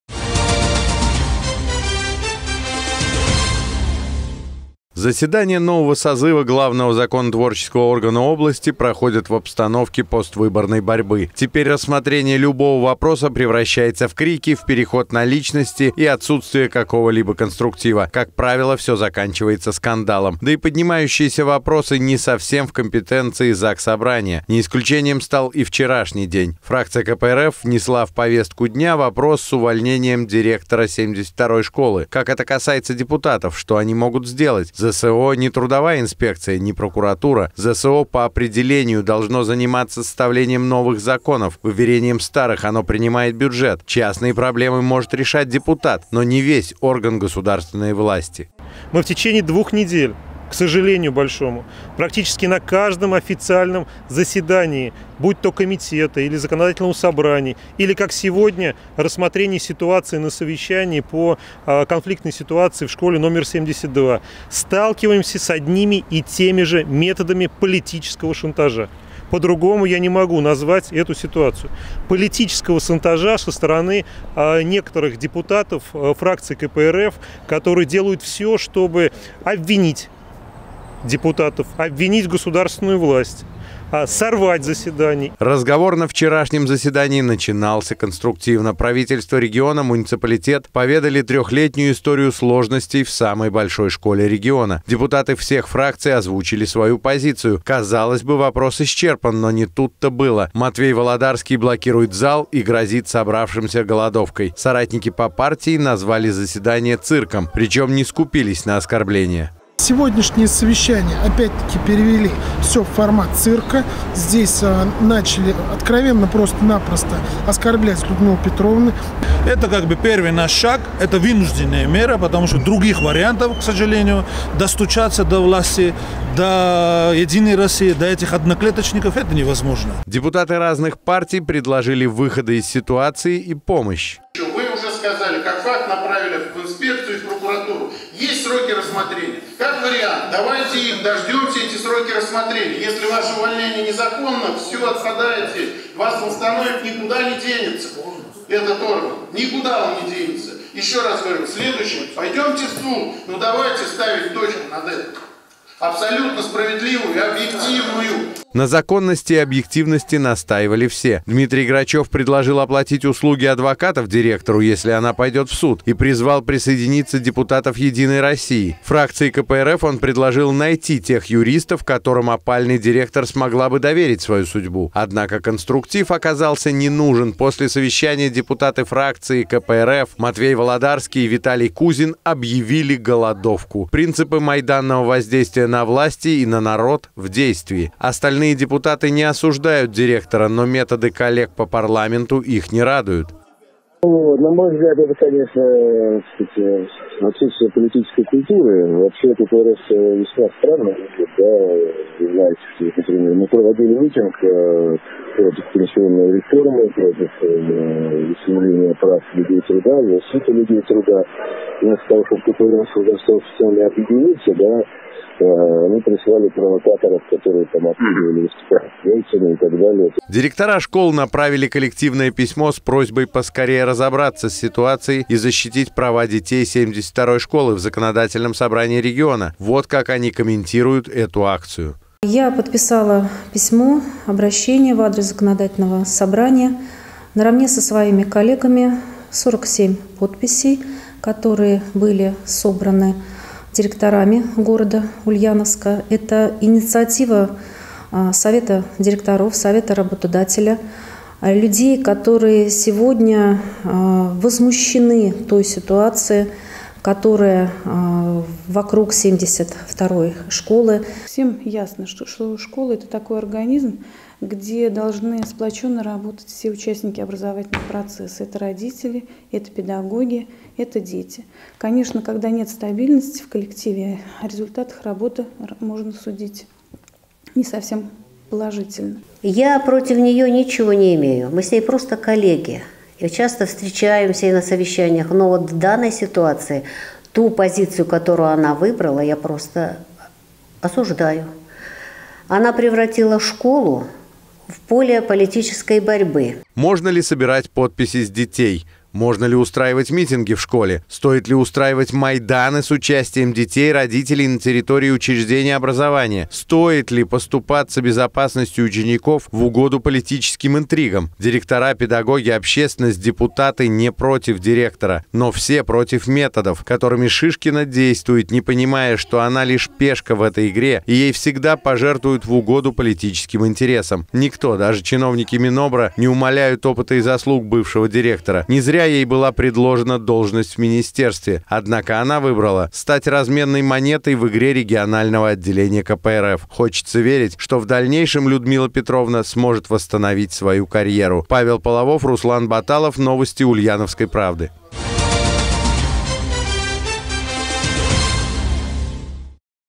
В Заксобрании решали школьные вопросы
Пока заседания нового созыва главного законотворческого органа Ульяновской области проходят в обстановке поствыборной борьбы. Теперь рассмотрение любого вопроса превращается в крики, переход на личности и отсутствие какого-либо конструктива. Как правило, все заканчивается скандалом. Да и поднимающиеся вопросы не совсем в компетенции Заксобрания. Не исключением стал и вчерашний день. Фракция КПРФ внесла в повестку дня вопрос с увольнением директора 72 школы.